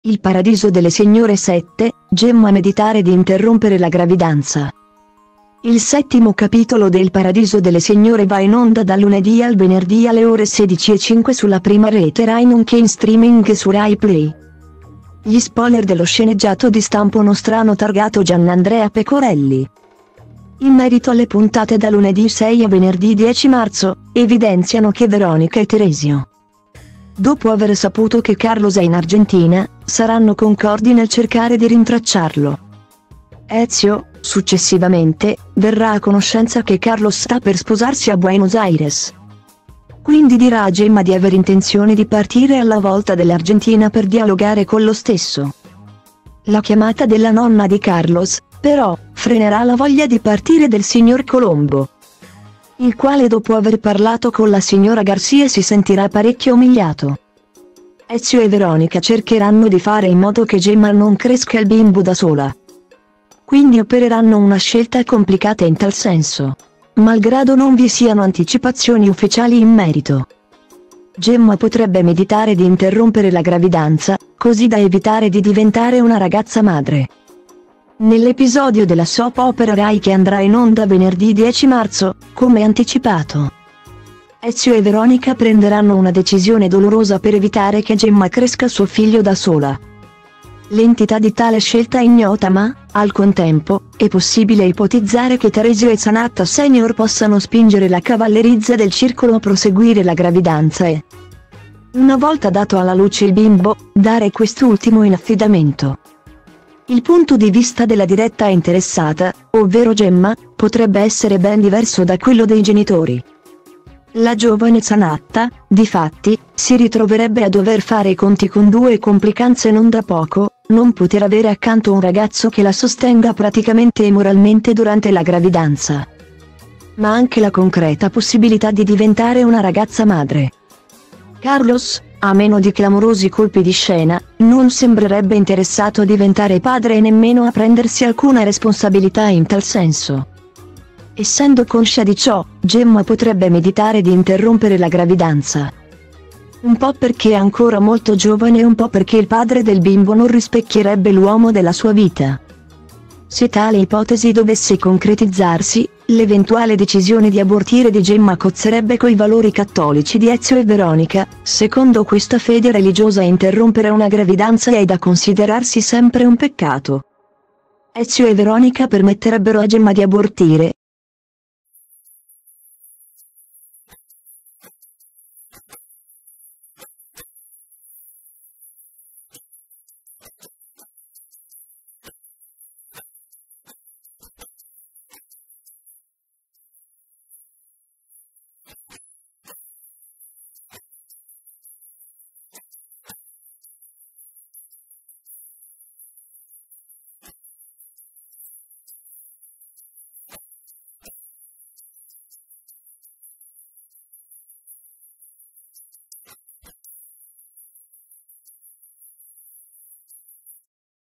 Il Paradiso delle Signore 7, Gemma meditare di interrompere la gravidanza
Il settimo capitolo de Il Paradiso delle Signore va in onda dal lunedì al venerdì alle ore 16:05 sulla prima rete Rai nonché in streaming su RaiPlay.Gli spoiler dello sceneggiato di stampo nostrano targato Giannandrea Pecorelli, in merito alle puntate da lunedì 6 a venerdì 10 marzo, evidenziano che Veronica e Teresio,